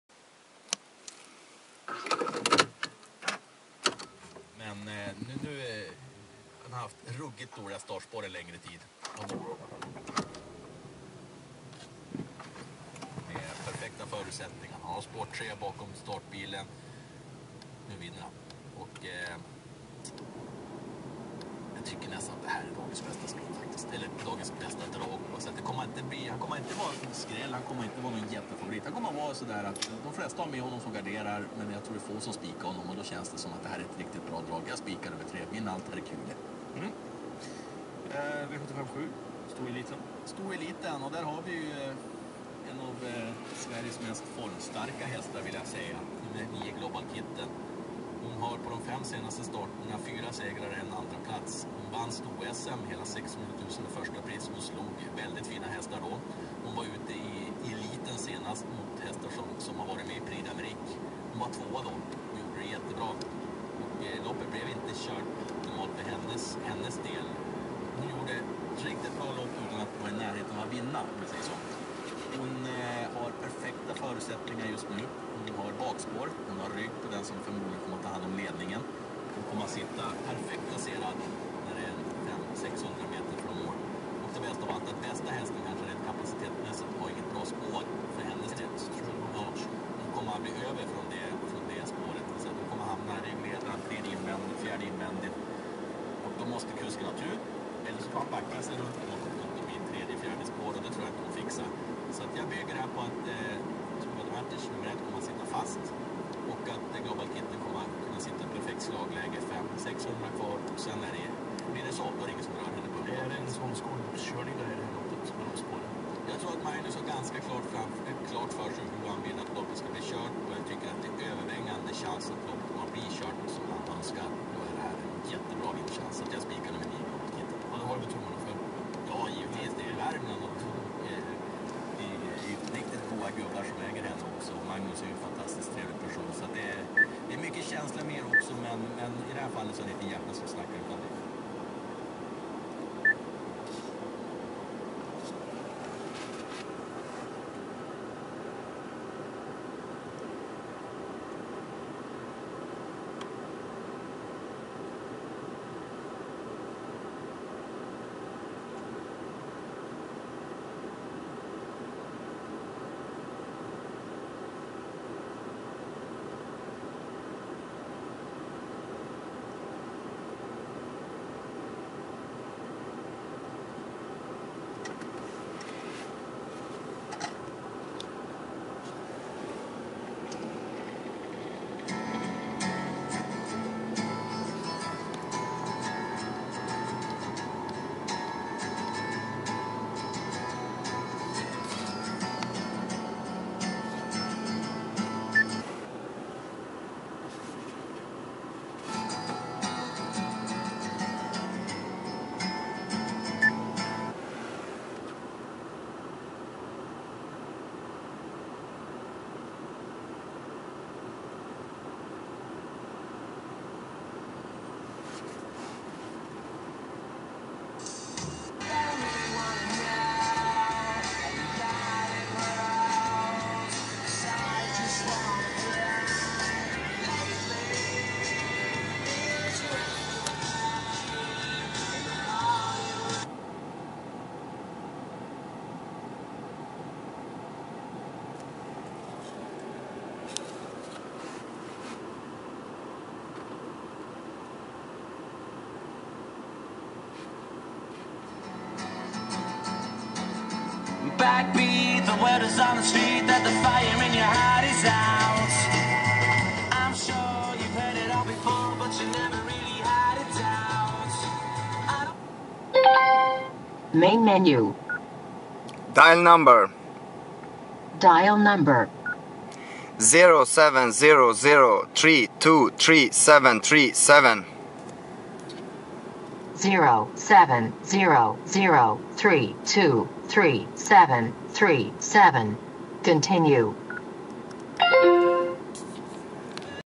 Hur du parar ihop telefonen med Original (OEM) Handsfree i Volkswagen Passat
Hur du ansluter mobiltelefonen (iPhone visad) till en eftermonterad Volkswagen bluetooth handsfree. Saknar du mobilförberedelsen i din VW är det möjligt att eftermontera original bluetooth systemet. Vänligen kontakta oss för mer information.

- Behåll din bil original